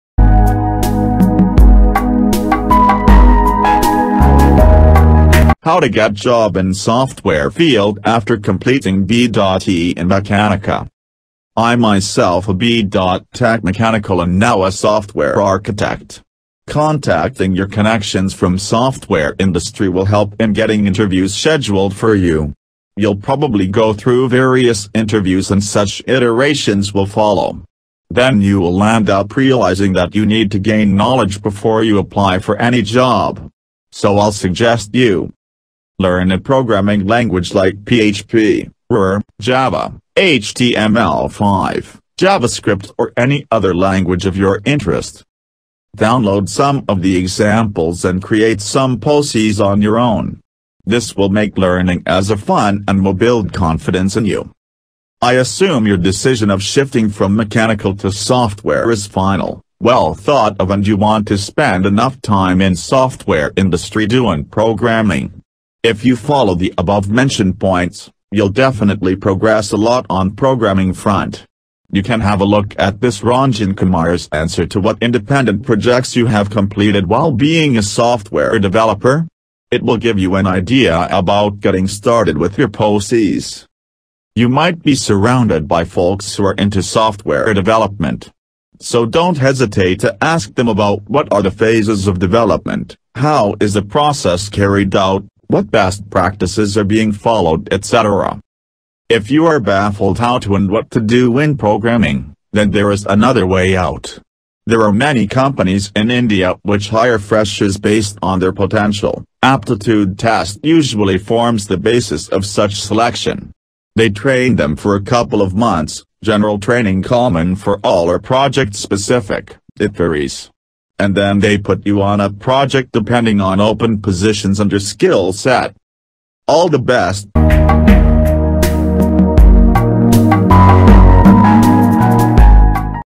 How to get job in software field_for mech engineer JOBS & CAREER
How to get job in software field after completing B.E in mechanical
I'm myself a B.Tech Mechanical and now a software architect. Contacting your connections from software industry will help in getting interviews scheduled for you. You'll probably go through various interviews and such iterations will follow. Then you'll land up realizing that you need to gain knowledge before you apply for any job. So I'll suggest you:

Learn a programming language like PHP, RoR, Java, HTML5, Javascript or any other language of your interest.
Download some of the examples and create some PoCs on your own. This will make learning as a fun and will build confidence in you.
I assume your decision of shifting from mechanical to software is final, well thought of and you want to spend
enough time in software industry doing programming.
If you follow the above mentioned points, you'll definitely progress a lot on programming front.
You can have a look at this Ranjan Kumar's answer to What independent projects you have completed while being a software developer?  It will give you an idea about getting started with your PoCs.
You might be surrounded by folks who are into software development. So don't hesitate to ask them about what are the phases of development, how is the process carried out, what best practices are being followed etc.
If you are baffled how to and what to do in programming,
then there is another way out.
There are many companies in India which hire freshers based on their potential
(Aptitude test usually forms the basis of such selection).
They train them for a couple of months (general training common for all or project-specific, it varies).
And then they put you on a project depending on open positions and your skill set. This video is all about to get emploied in companies and grow up in life...
do share this...